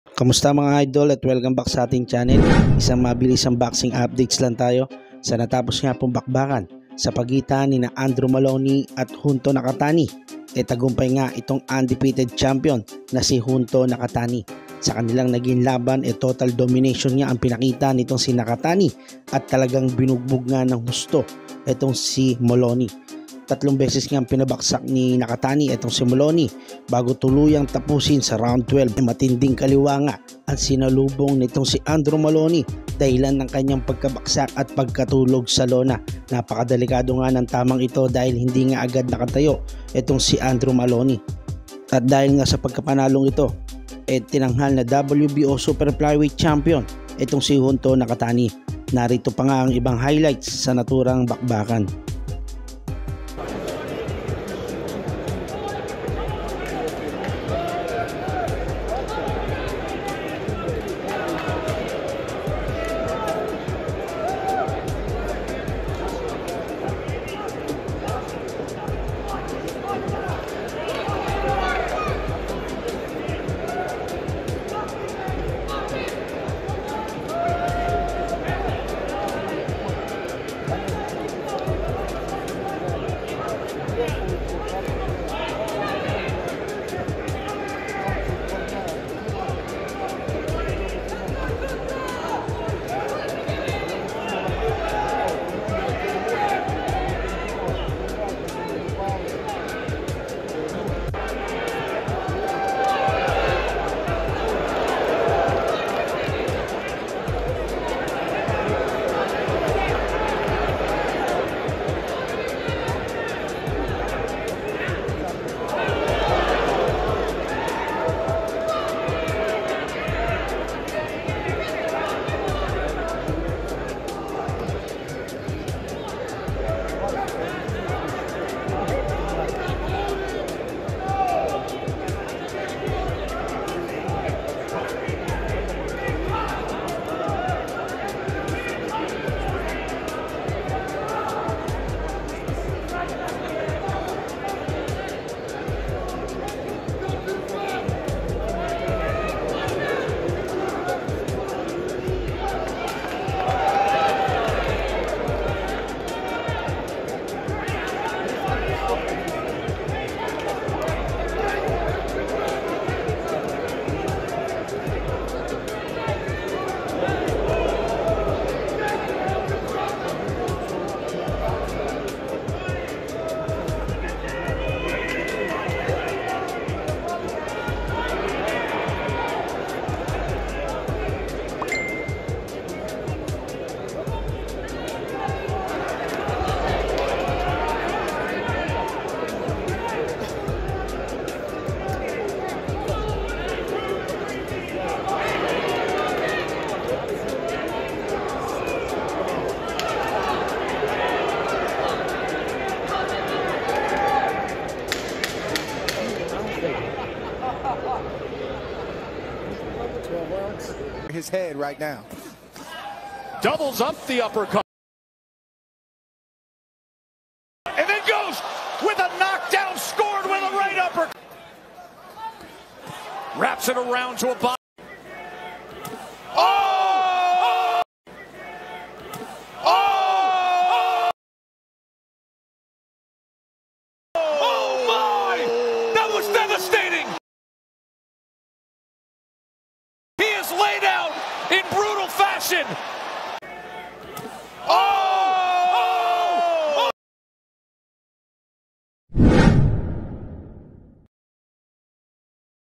parang hatton ang nangyari ! Junto Nakatani dinurog si Adrew Maloney !
Parang Hatton ang nangyari!Junto Nakatani dinurog si Adrew Moloney